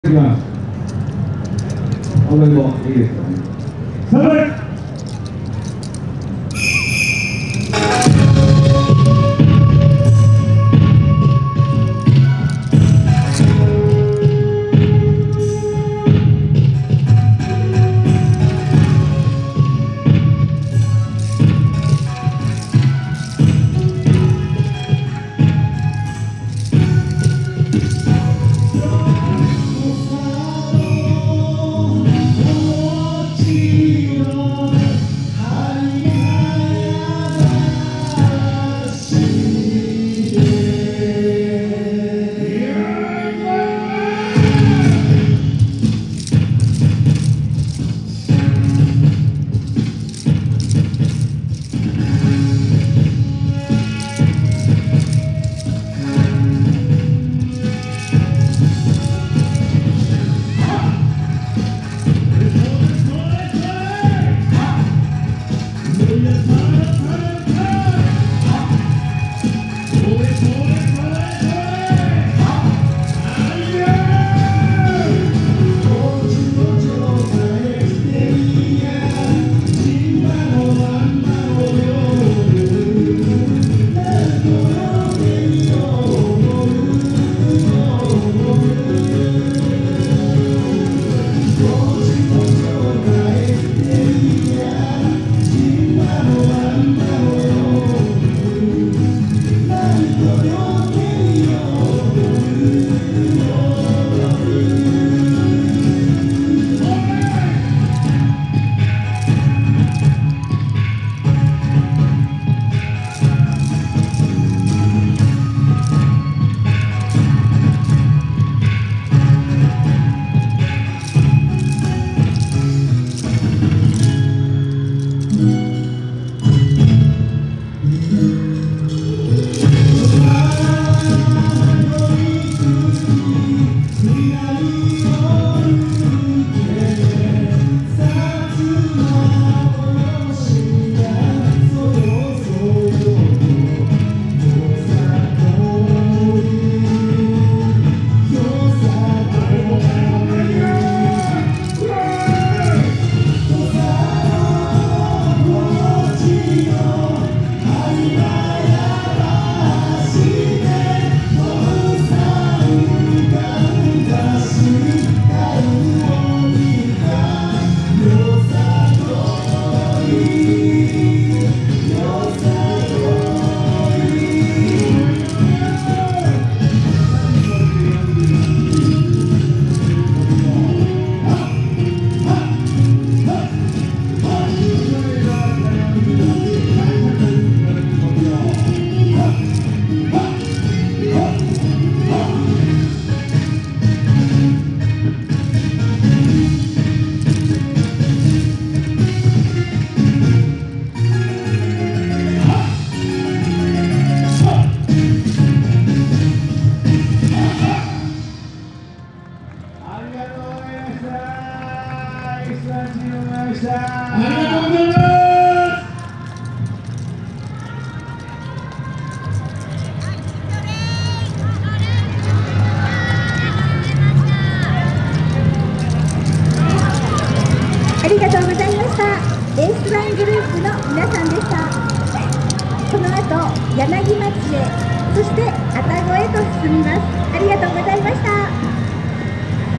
[4K] エースワングループ 高知よさこい祭り 2023 本祭2日目
2023年8月11日(金)
第70回高知よさこい祭り　本番2日目